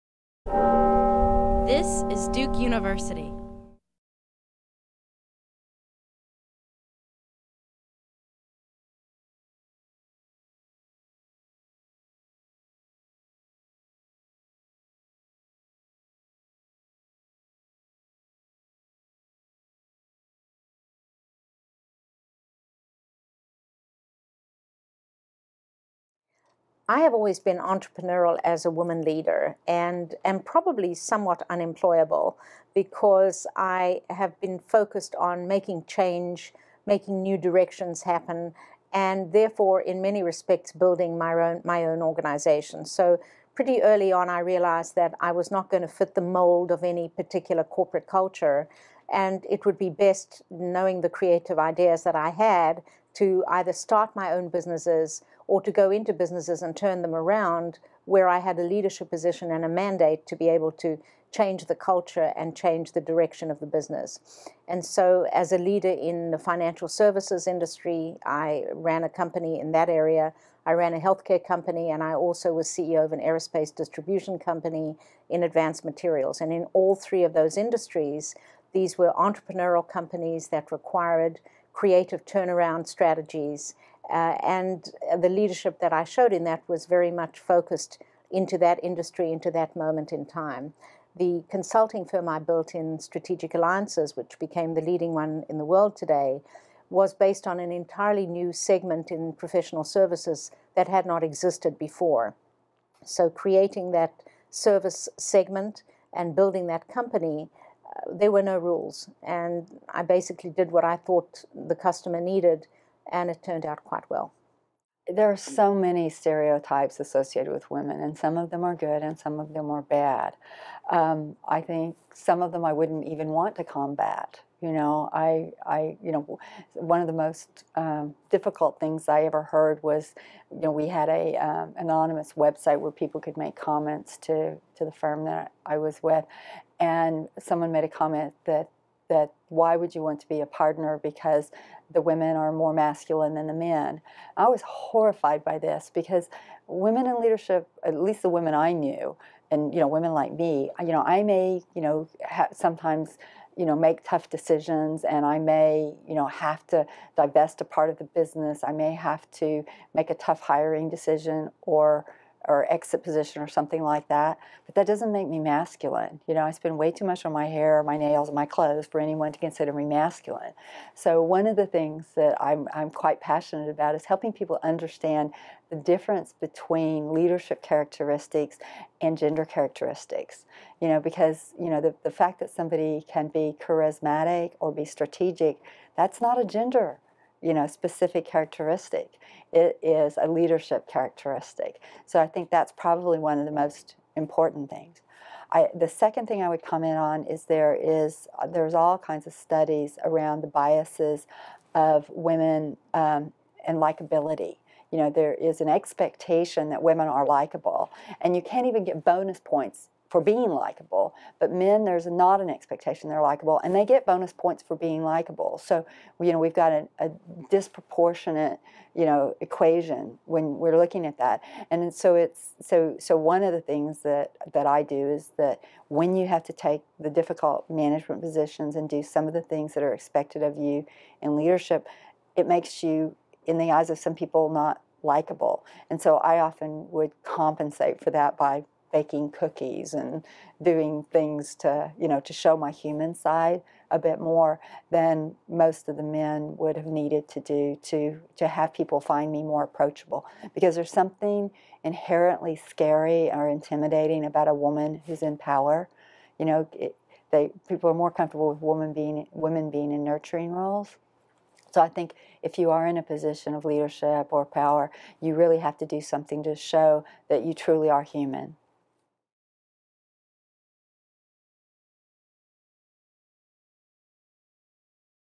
A Discussion on Gender Stereotypes in the Workplace
Larraine Segil (Board Member, Frontier Communications and Foundation Chair, Committee of 200) and Jan Babiak (Board Member, Walgreens, Bank of Montreal and Royal Mail Group) discuss gender stereotypes in the workplace. Hear them explain how powerful women can be leaders in their own right, run a business without being called masculine, and make tough decisions while being seen as likeable.